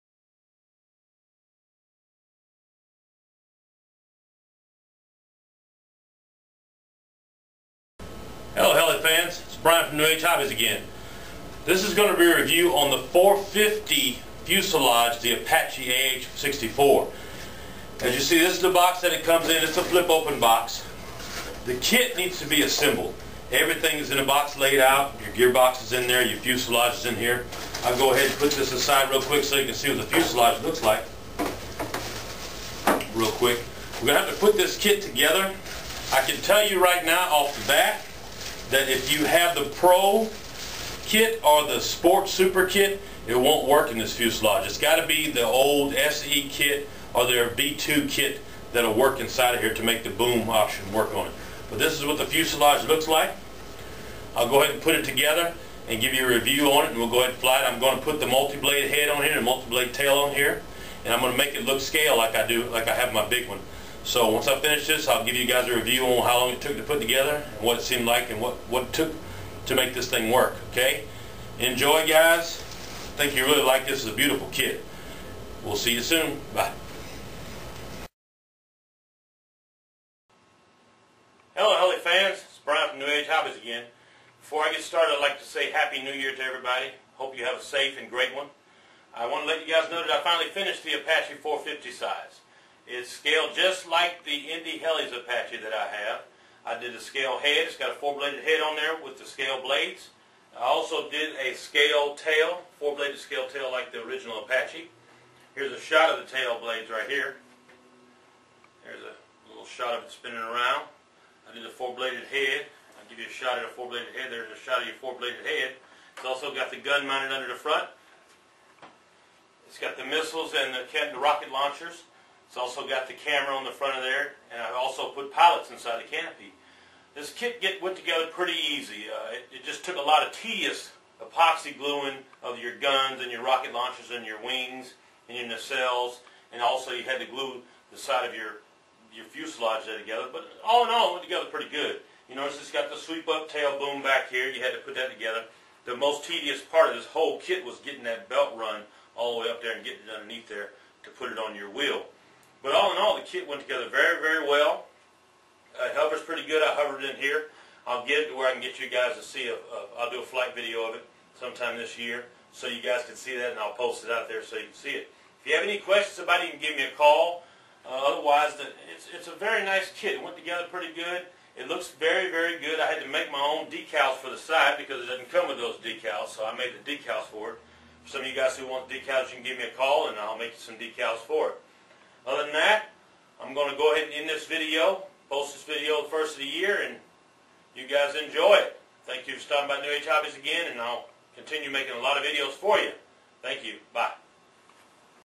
Out-of-Box Review 450 Apache fuselage build
This is an Out-of-Box Review of the 450 Apache AH-64 fuselage build. I built it to scale with 4 blade head and x 4 blade tall.